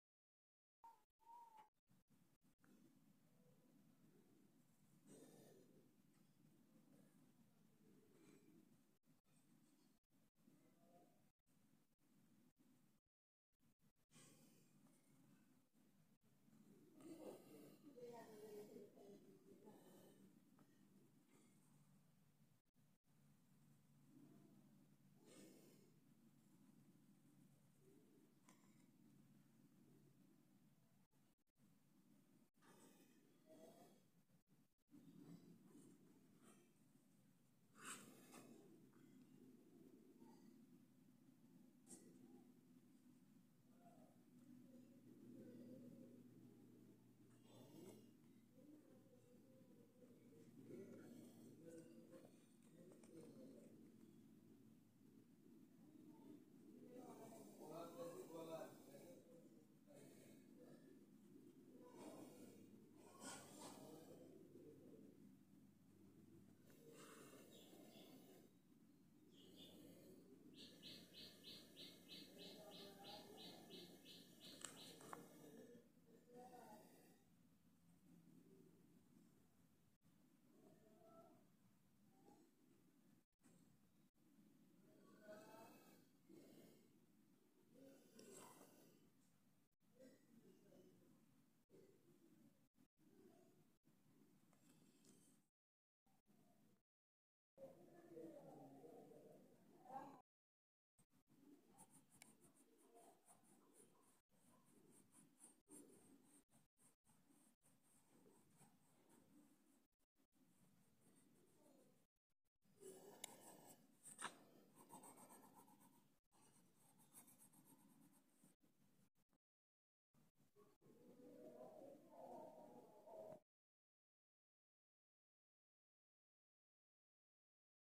কি ভাবে এক টানে আল্লাহ লেখা আর্ট করতে হয়(How to art name of Allah easy step by step)
How to art name of Allah easy step by step  for learn and art/ panting Tutorial easy way. Videos drawing lesson for all. Pencil drawing /colour /penting etc if my videos like